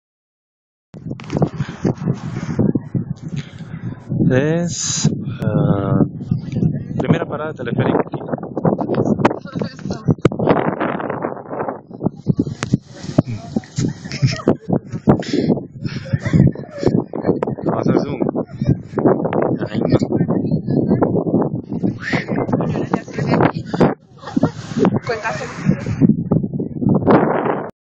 Teleférico de Quito - Vista General
Una vista 360° desde la montaña tras llegar en el Teleférico de Quito.